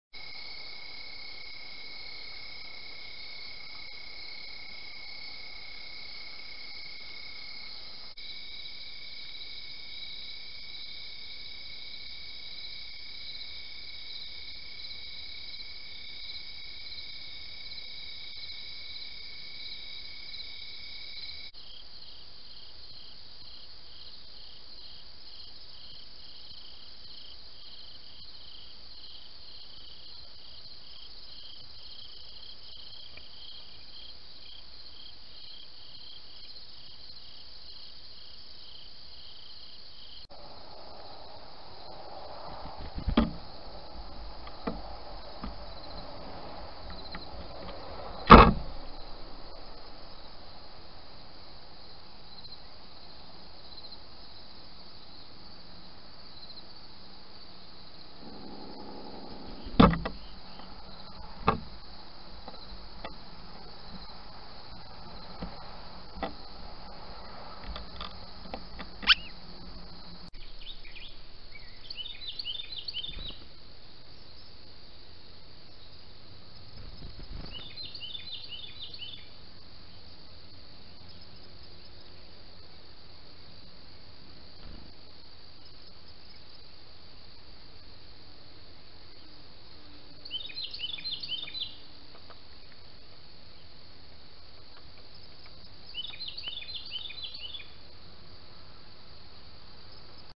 Opossum Climbs a Tree & Some Curious Birds
These videos were recorded on September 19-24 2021.
I set my trail camera on a tall pole to see if I could record one of the Opossums climbing the tree and I did. Also, Chickadees and a Tufted Titmouse were curious of the camera.